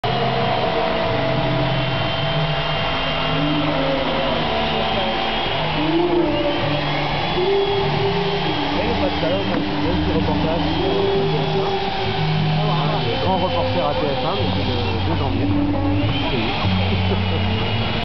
Bercy 2008 - Ice Age in Ice
Sculpture sur glace des personnages de Walt Disney